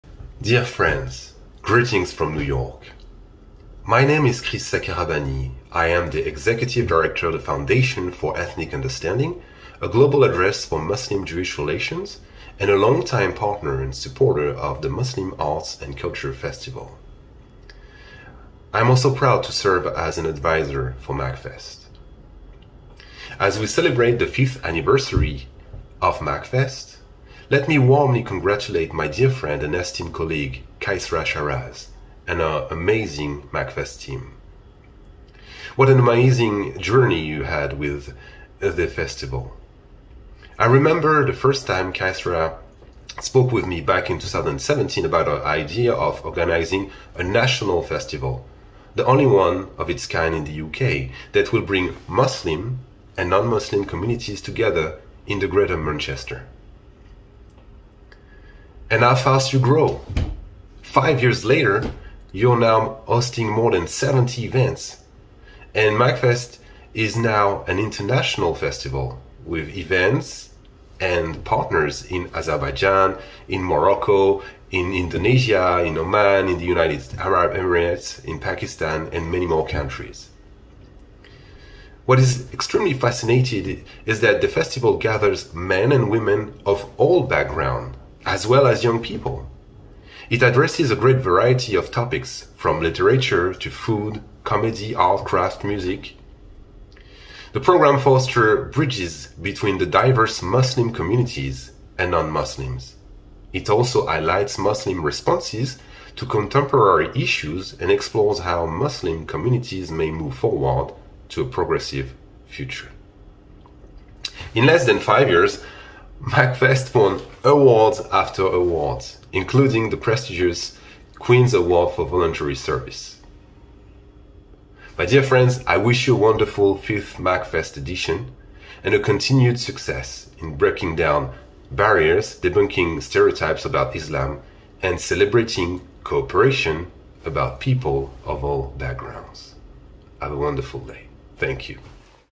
FFEU congratulatory message to MACFEST 5th anniversary celebration
FFEU President Chris Sacarabany sent a congratulatory message on the occasion of the 5th anniversary celebration of the Muslim Arts and Culture Festival 2023 (MACFEST) curated by Qaisra Shahraz.